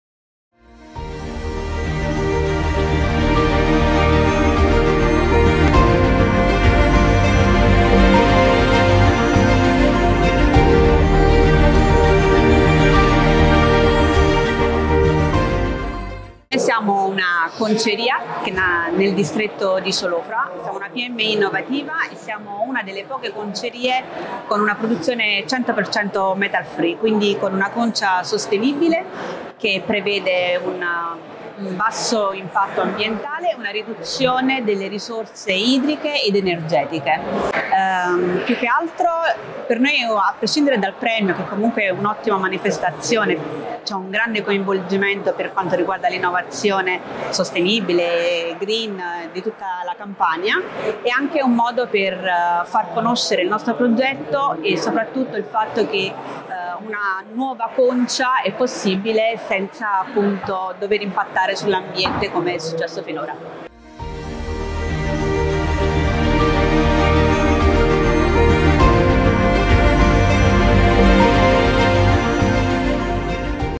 Premio Best Practices XVI Edizione
L'intervista ad Annalisa De Piano di Be Green Tannery